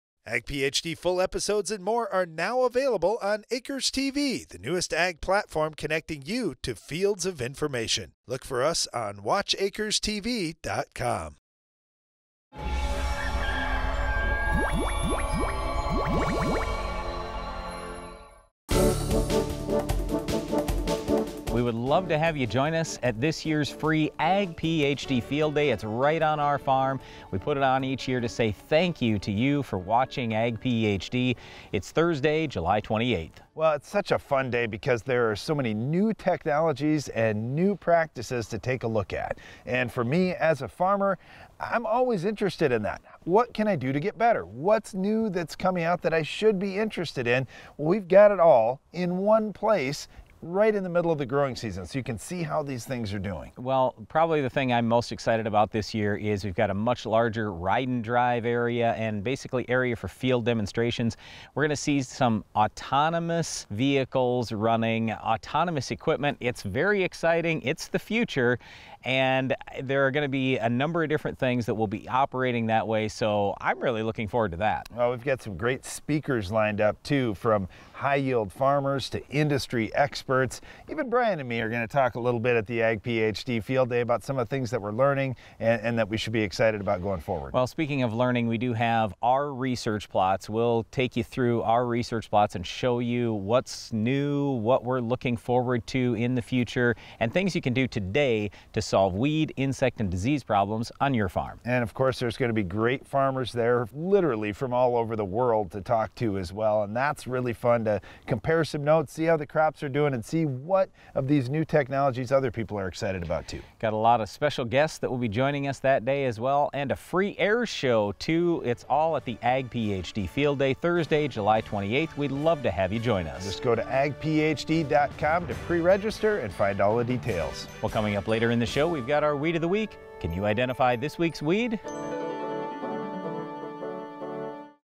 Ag PhD Field Day 2022
Farm Basics from Ag PhD Episode #1267 | Air Date 07/17/22 - Brian & Darren invite to come to Baltic, SD on Thursday 28th July 2022 for the free farm festivities on the Hefty fields.